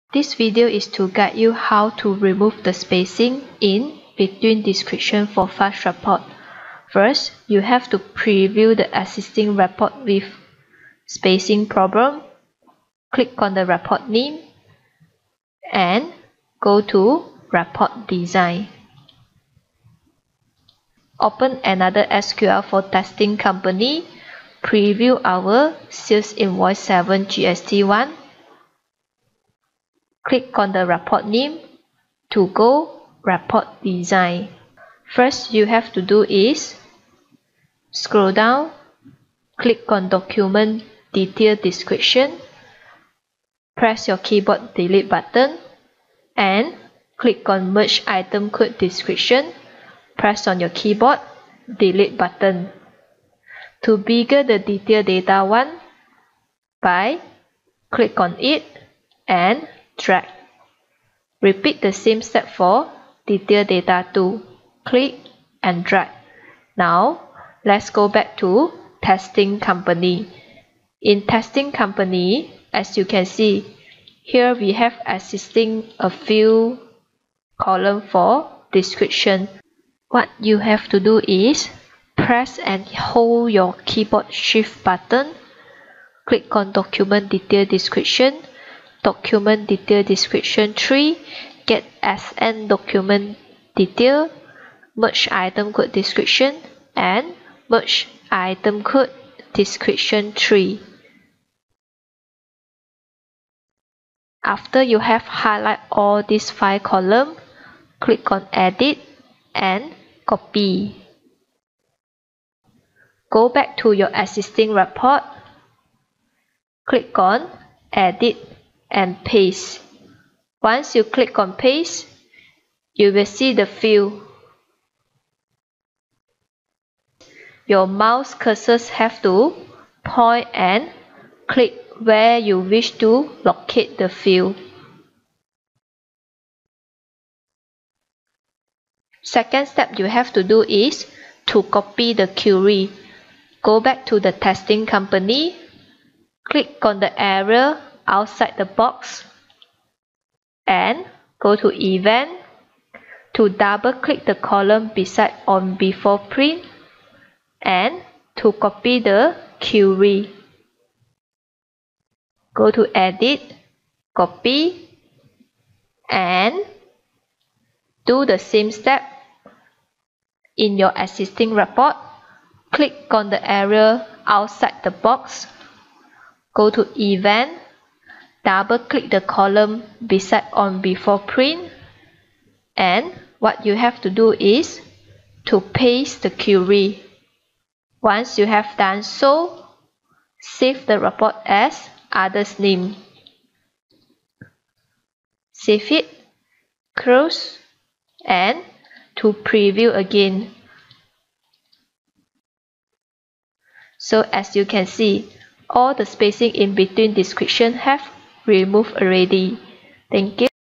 SQL ACCOUNT SOFTWARE | Guideline To Solve Spacing Between Description For Fast Report | IPOHONLINE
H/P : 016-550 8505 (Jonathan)
H/P : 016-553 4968 (Ong Yan)